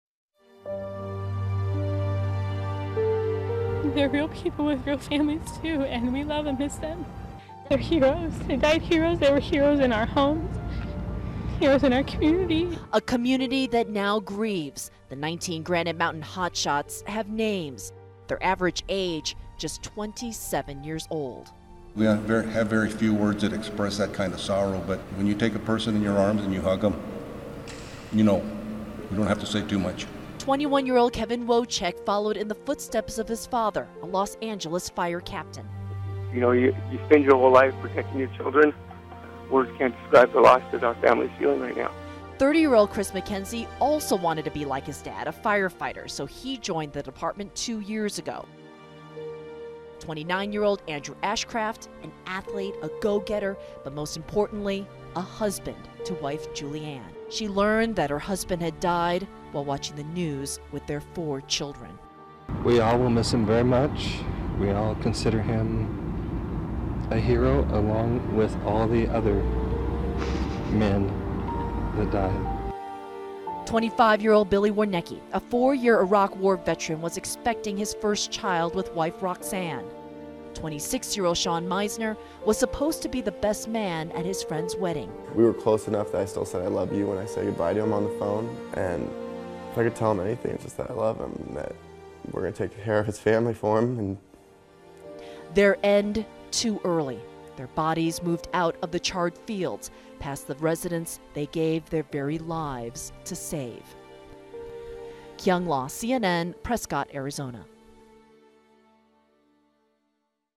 Community grieves for Hotshot firefighters killed in Arizona
CNN's Kyung Lah looks at the lives of the Hotshot firefighters from Arizona.